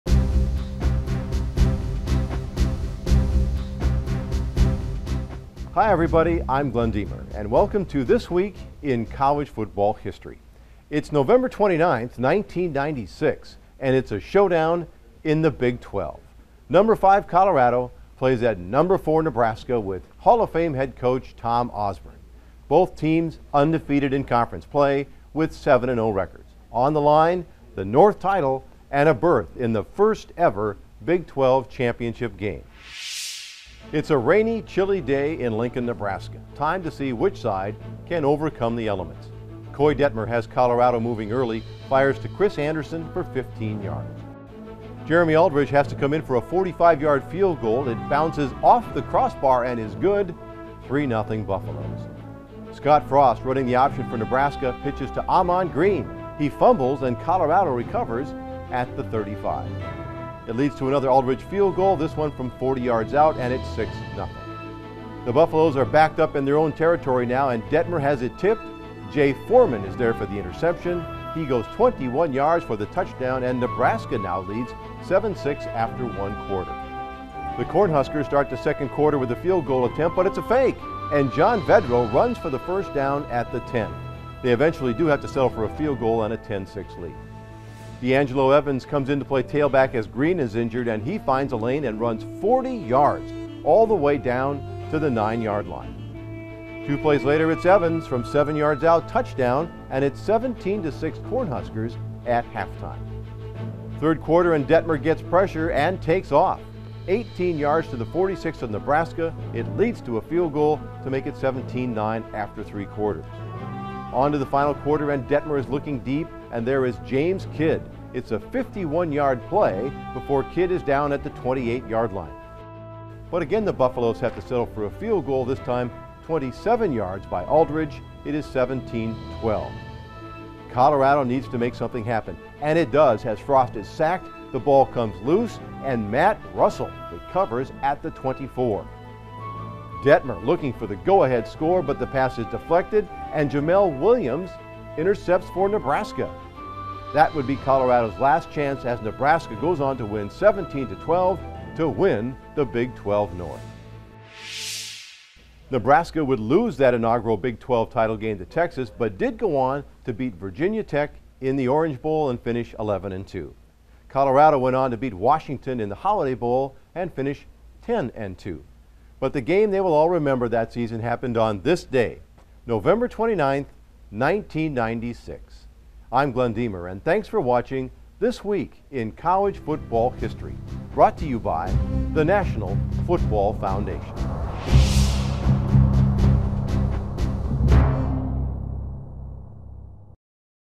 This Week in College Football History - Colorado vs Nebraska
Nov. 29, 1996 - Colorado vs Nebraska. Glen Dehmer has highlights from this Big 12 showdown. Nebraska endures and earns an invite to the first ever Big 12 Championship game as the Big 12 North champs.